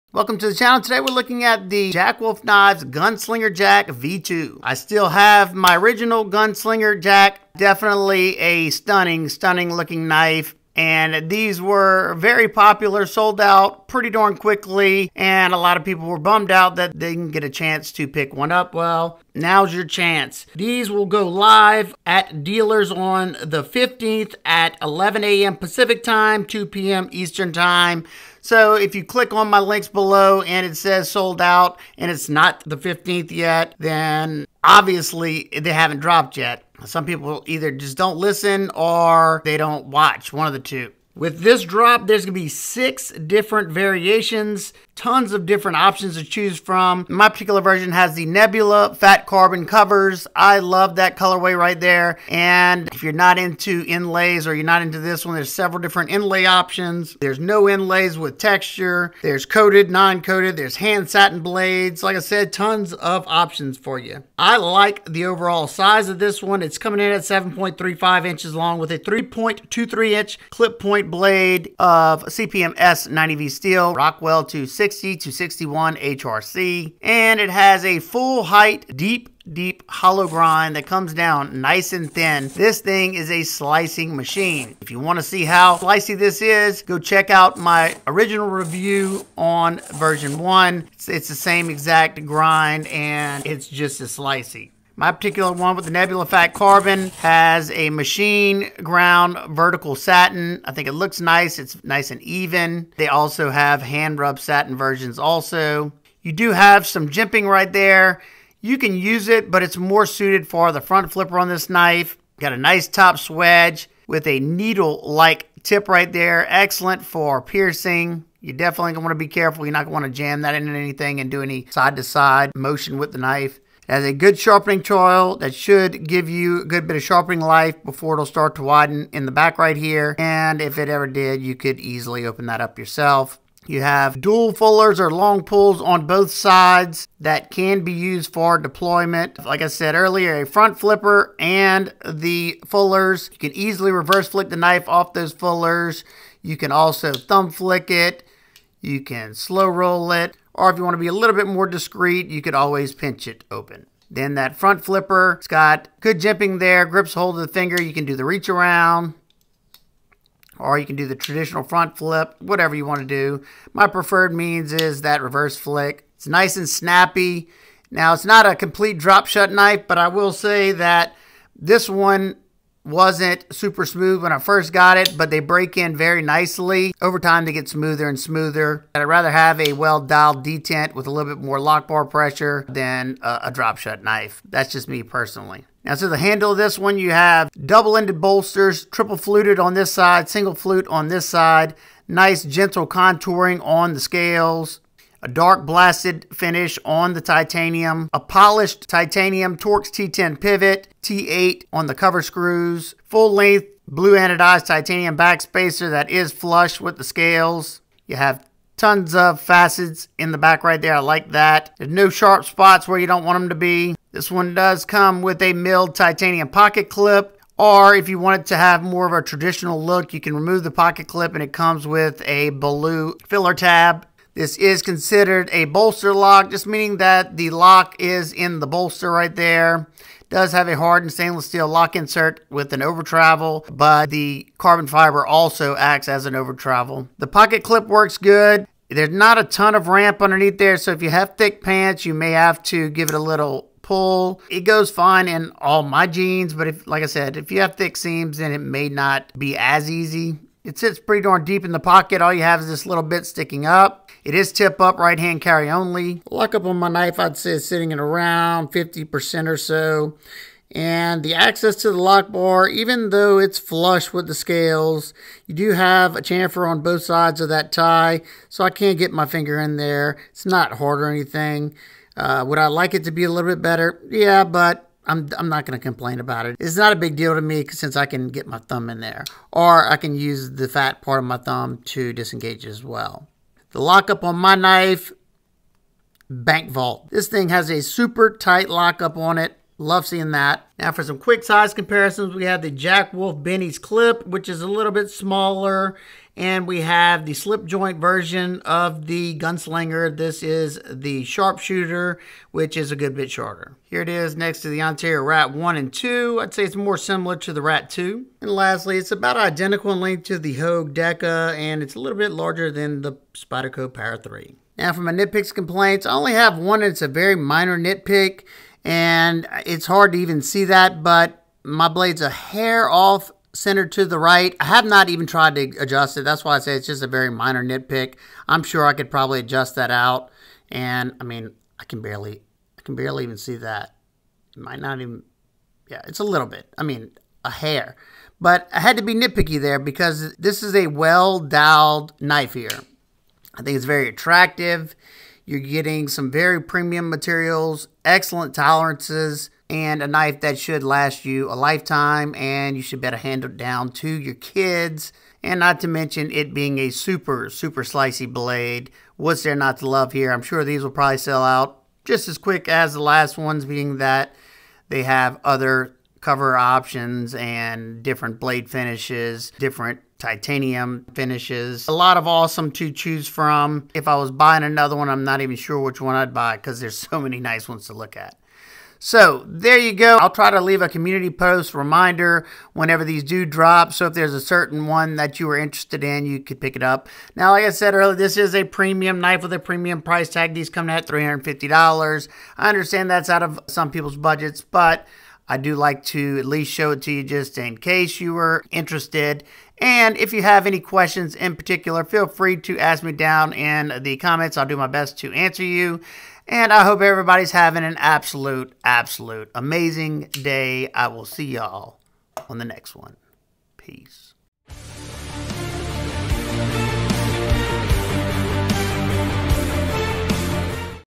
At Last The Return of the Jack Wolf Knives Gunslinger
At Last The Return of the Jack Wolf Knives Gunslinger. This was a very popular model for Jack Wolf Knives and it sold out rather quickly. This time there are more options available plus more knives available. In this video we discuss the what’s good and what’s bad as well as doing some real world testing to see how it performs. My hope is that by the end of this video your able to make a informed decision on whether or not you should buy this knife or not.

~My Most Reccomended Budget Knives

~My Recommended Knife Maintenance Tools